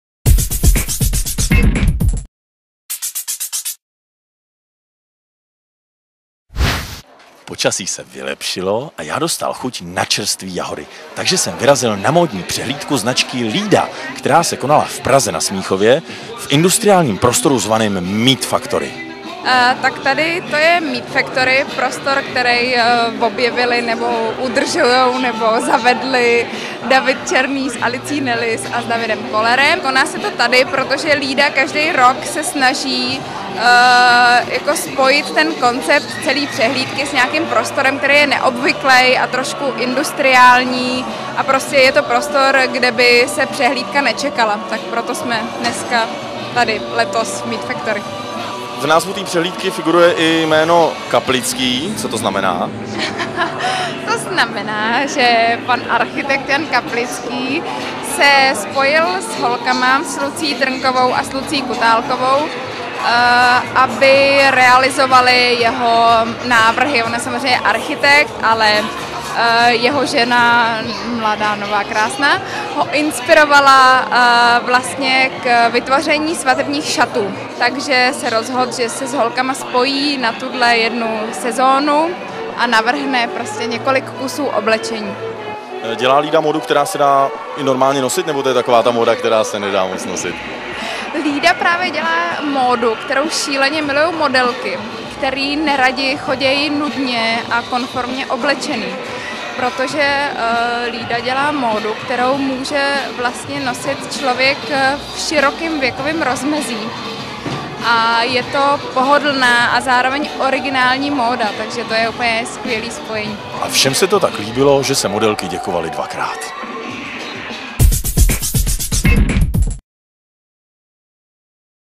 Modelky z fabriky
Modelky a plavky, to bylo pro Gustava něco! A zajímavější to bylo o to, že plavky navrhoval taky Jan Kaplický.